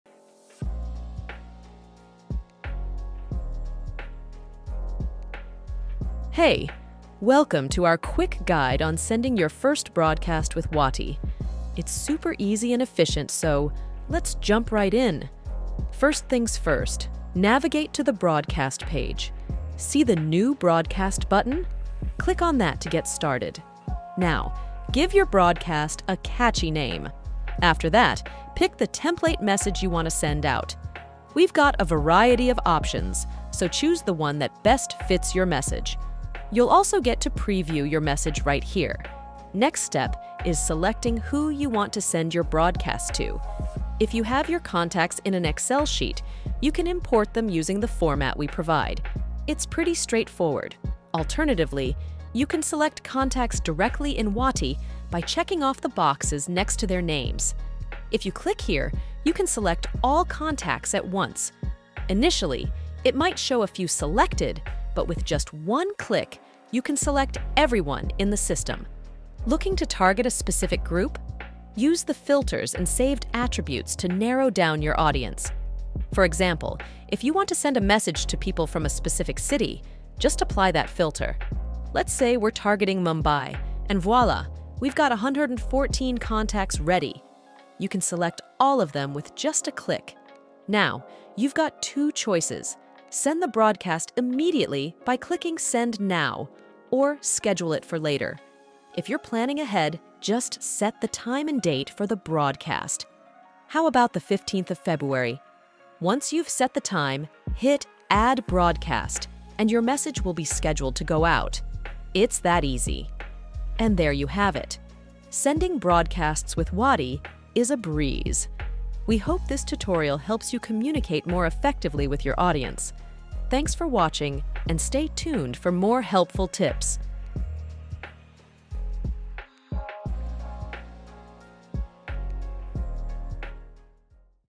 How to Start Your First WhatsApp Broadcast with Wati?
Ready to take your WhatsApp communication to the next level? Our tutorial provides a step-by-step guide on how to send your first broadcast message using Wati. Ideal for marketers, business owners, and anyone looking to engage with a broad audience, this video covers everything from creating a catchy broadcast name to scheduling your messages for maximum impact. Learn the ins and outs of efficient broadcast messaging, how to import contacts, select the proper message templates, and target specific customer segments with Wati’s easy-to-use platform. Whether you're broadcasting immediately or planning for a future date, Wati’s intuitive interface makes it simple. Watch now to master the art of WhatsApp broadcasts with Wati and enhance your audience communication strategy.

Timestamps:

[0:06 - 0:15] Introduction to Wati Broadcasts
[0:16 - 0:24] Creating a Broadcast Name
[0:25 - 0:36] Choosing the Right Template Message
[0:37 - 0:49] Importing Contacts and Selecting Recipients
[0:50 - 1:00] Using Filters for Targeted Broadcasting
[1:01 - 1:38] Sending Broadcasts Immediately or Scheduling for Later
[1:39 - 1:52] Confirming Broadcast Details and Scheduling
[1:53 - 2:03] Wrap-Up and Additional Tips